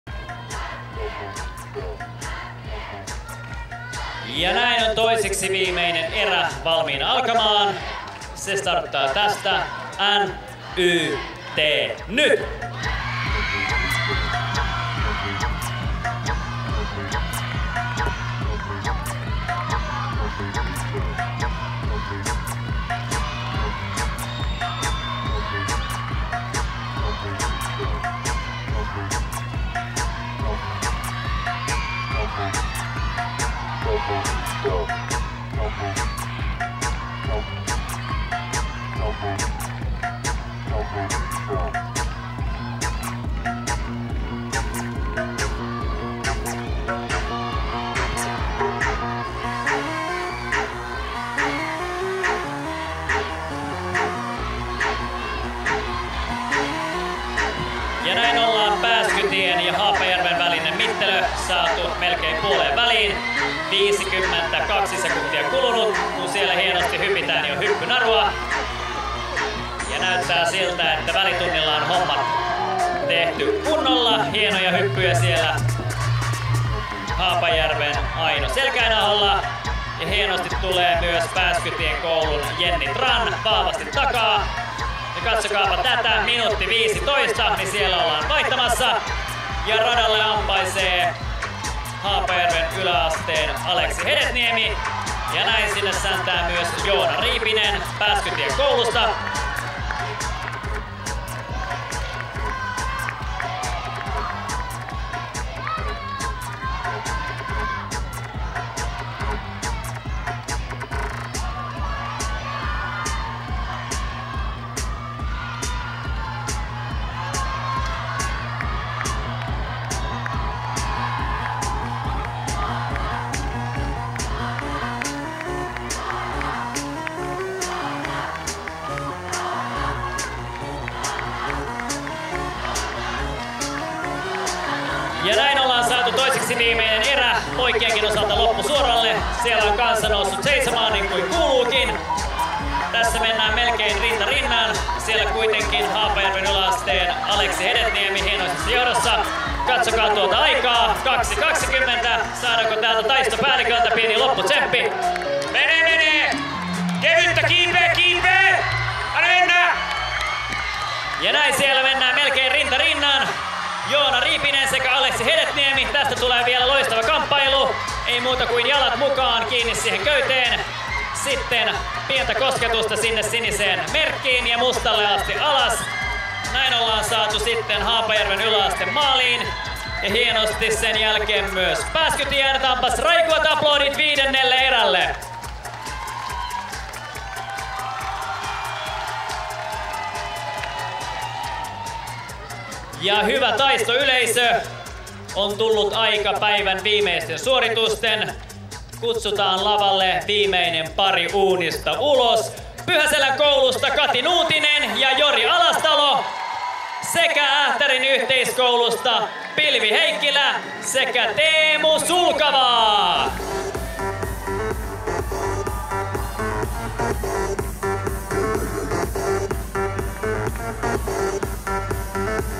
Taisto 2015 -ratafinaali: Pääskytien koulu vs. Haapajärven yläaste
Taisto 2015 -ratafinaalin viidennessä parissa kilpailivat Pääskytien koulun Jenni Tran ja Joona Riipinen ja Haapajärven yläasteen Aino Selkäinaho ja Aleksi Hedetniemi.